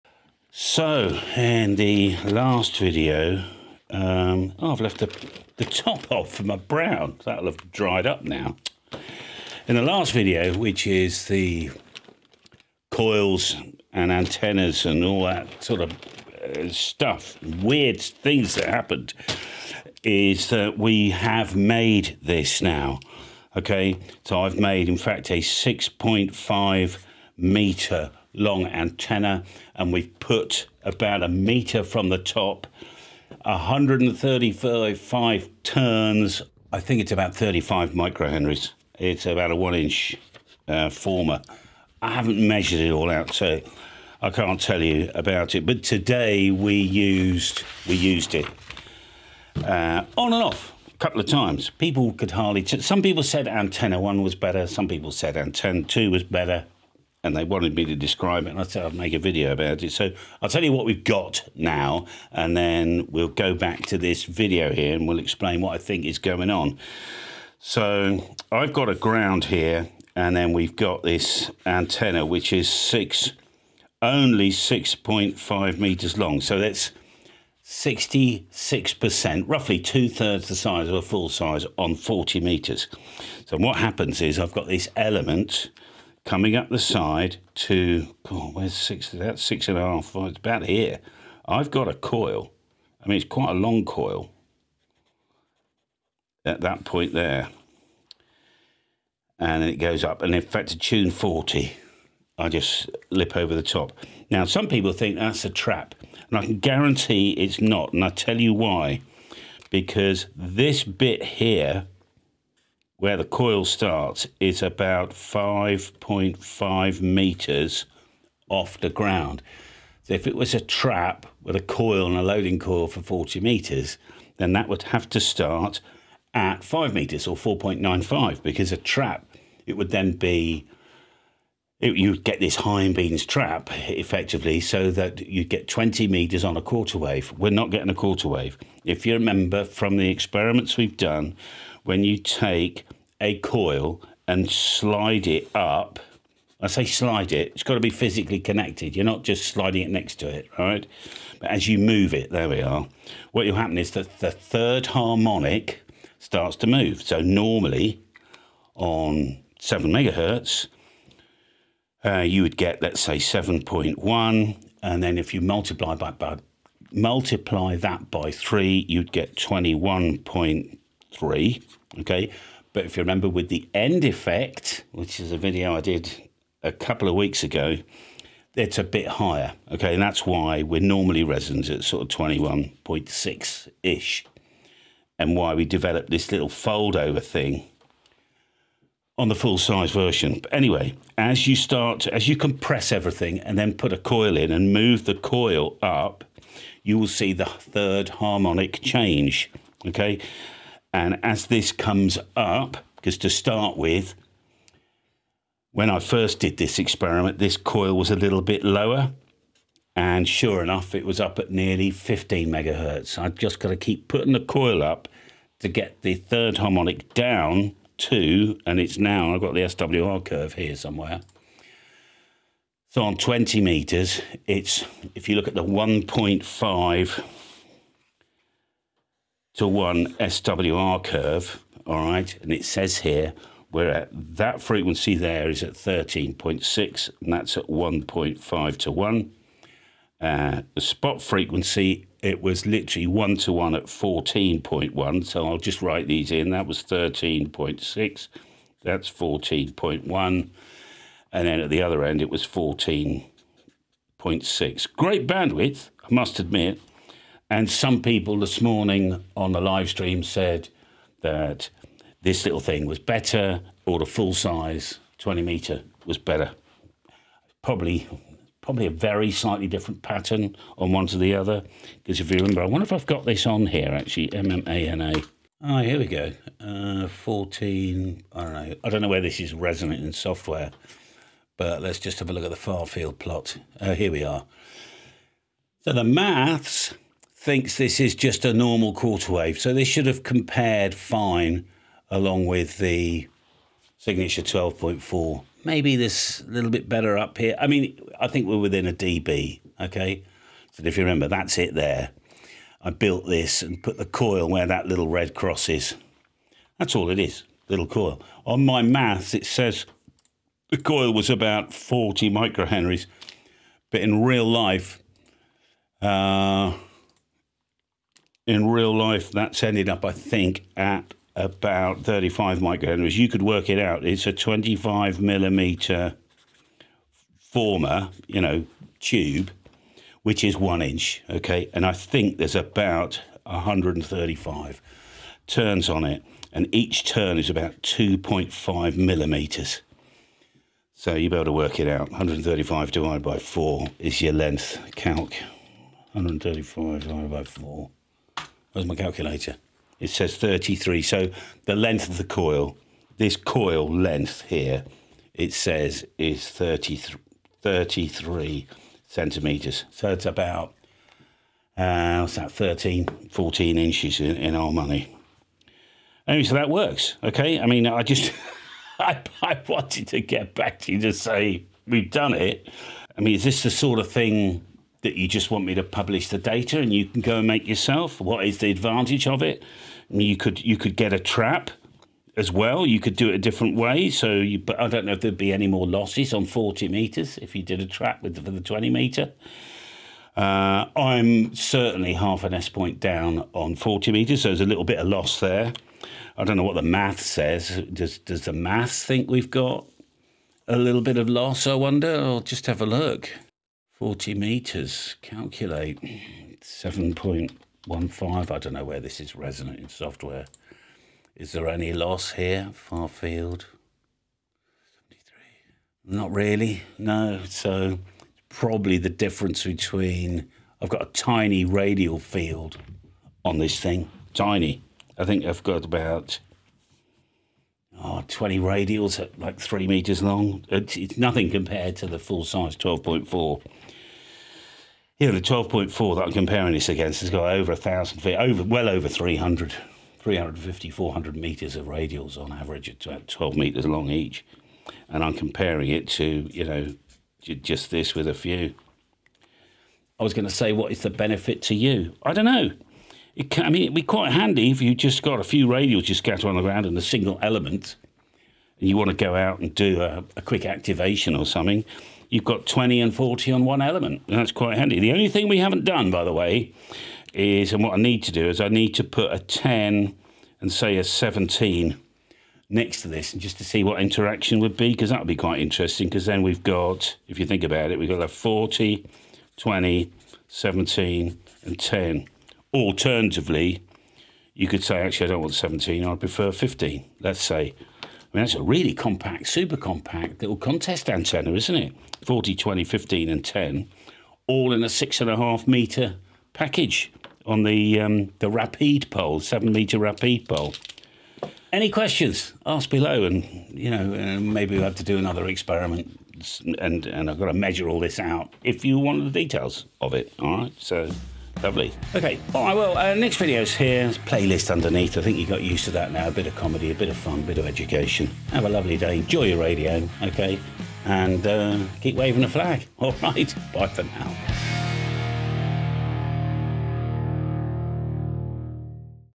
Success! 20m and 40m on same element - Amazing Result!
The placement of a coil on an element affects the third harmonic. If you can work out exactly where to put this coil, we can then force a harmonic on almost any band you like. In this example we have made a compact 40m antenna of only 6.5m long and then made 20m to work on the third harmonic with very little loss. See what you think. Callum.

- Live Stream where I Switched Between